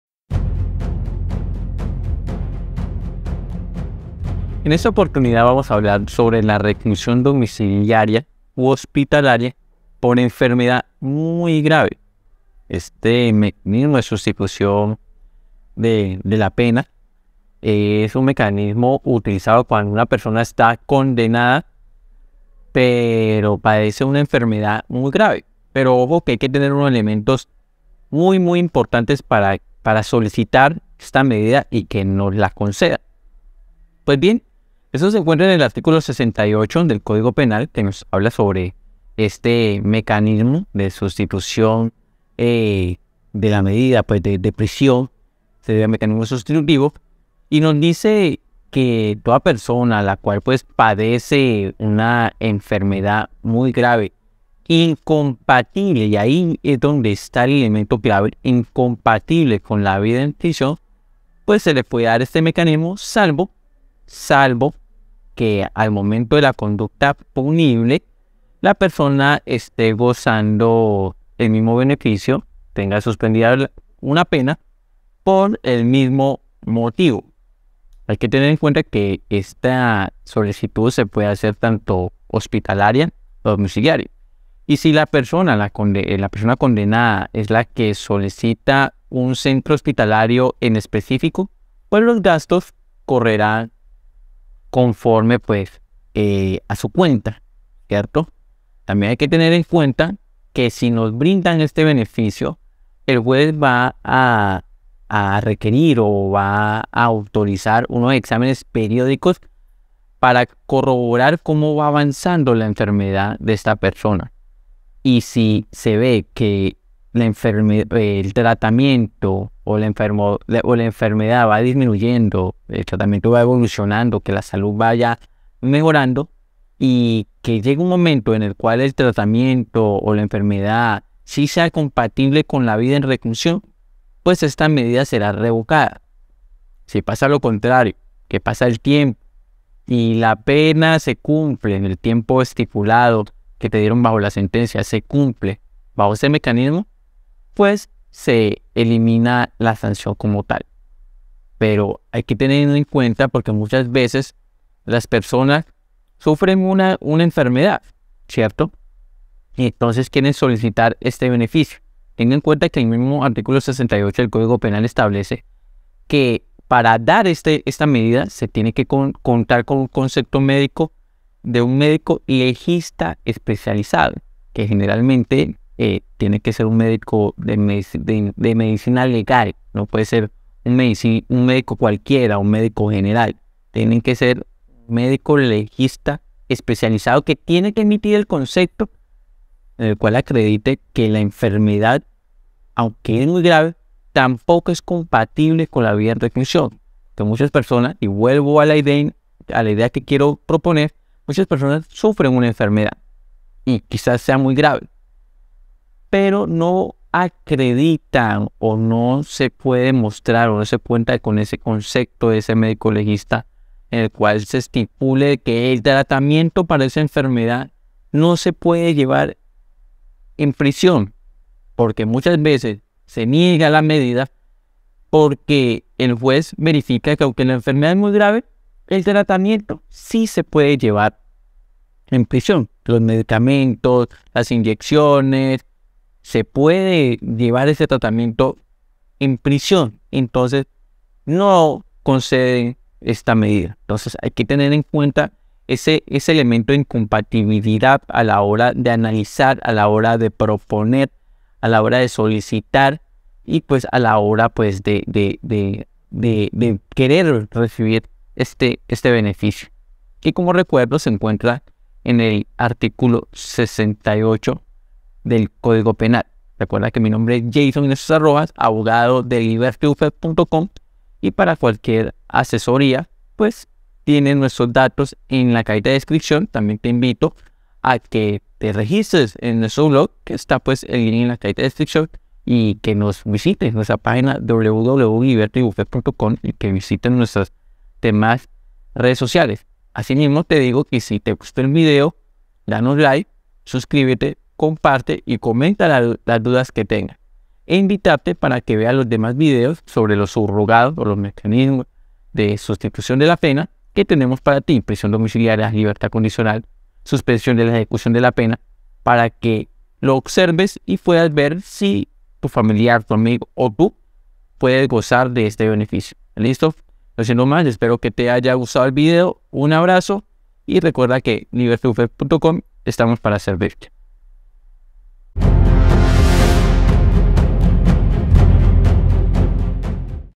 RECLUSIÓN DOMICILIARIA U HOSPITALARIA POR ENFERMEDAD MUY GRAVE EN COLOMBIA-JEICSON HINESTROZA ROJAS
¿tienes un amigo o un familiar condenado que se encuentra grave de salud? mira el siguiente video y entérate si puede acceder a este beneficio.